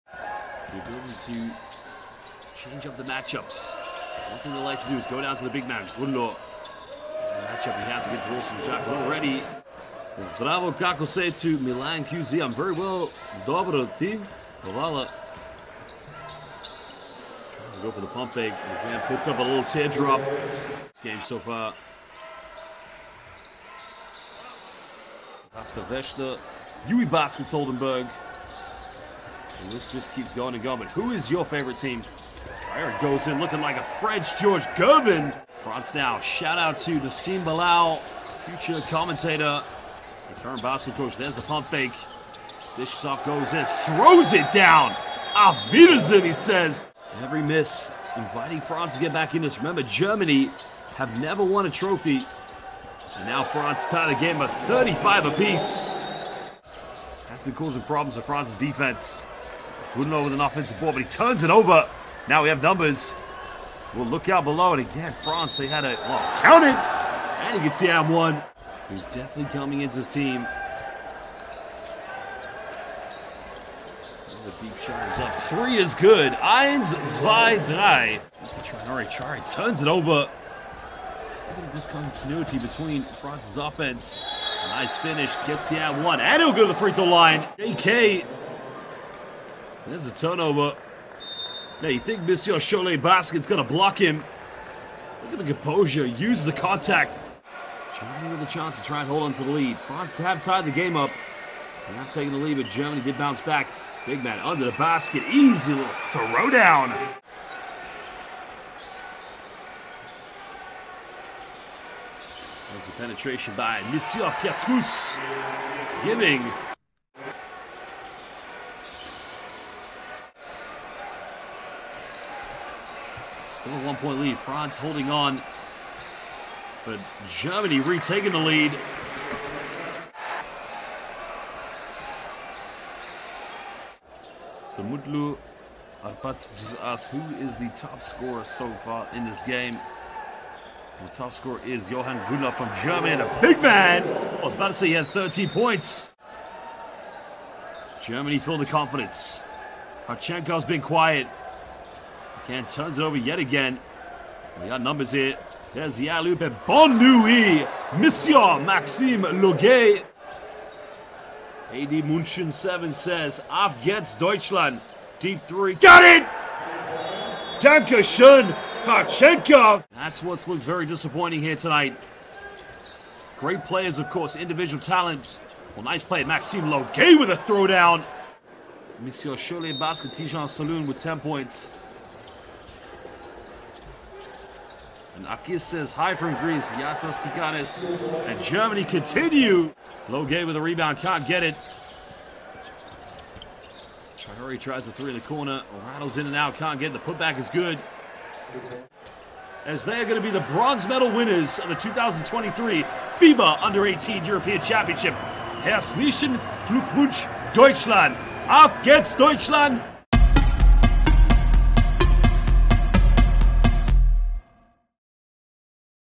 France 🇫🇷 vs Germany 🇪🇸 | 3rd Place Game Highlights | FIBA U18 European Championship 2023
Watch the highlights of the 3rd place game between France and Germany at the FIBA U18 European Championship 2023!